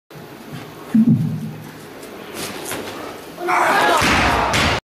Thimo Solar 3. Versuch Reißen 160kg
Videos der Bundesliga-Begegnung AC Mutterstadt gg. Germania Obrigheim im Gewichtheben vom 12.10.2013